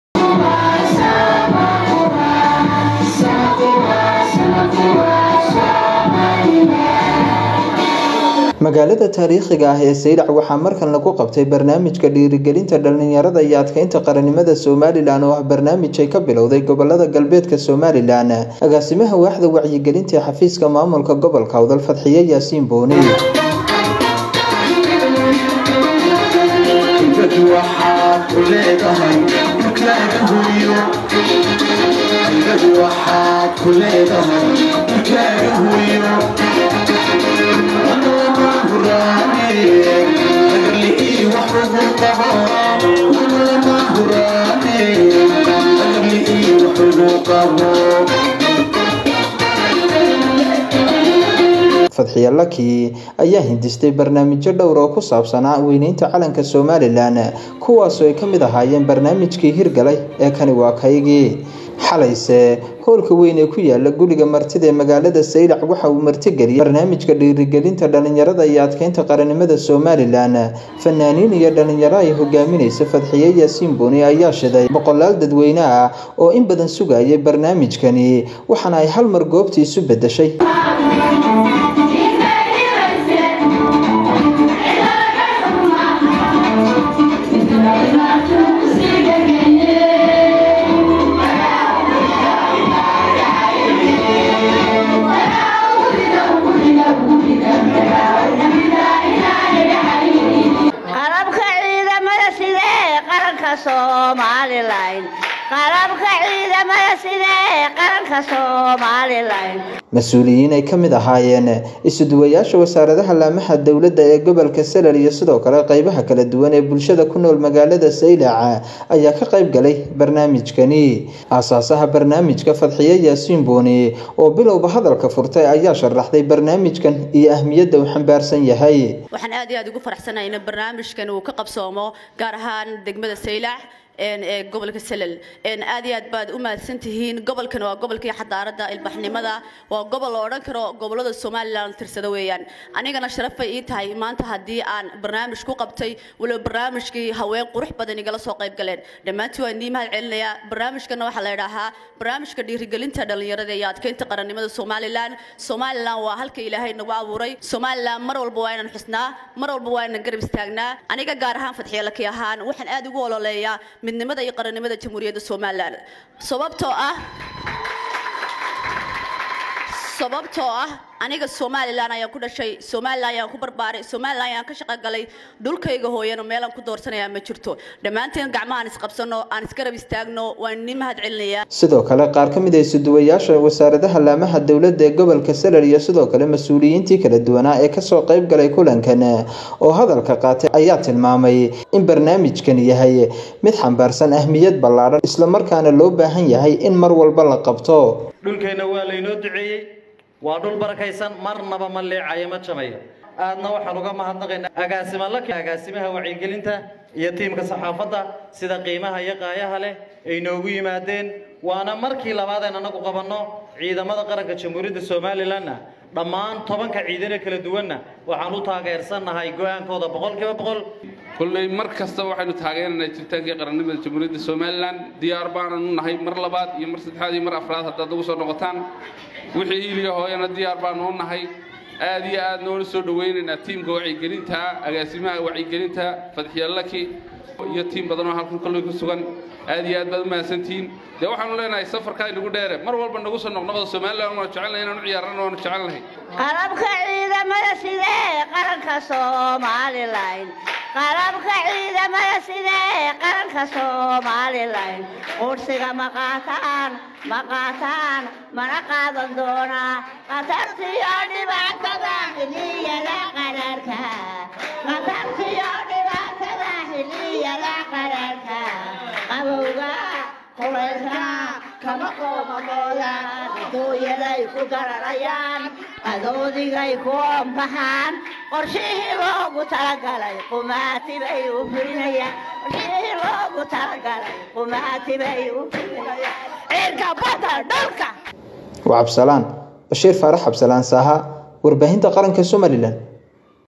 Magaalada Saylac oo lagu qabtay barnaamijka dhiiri-galinta dhalinyarada iyo adkaynta
Magaalada Saylac oo lagu qabtay barnaamijka dhiiri-galinta dhalinyarada iyo adkaynta qaranimada somaliland.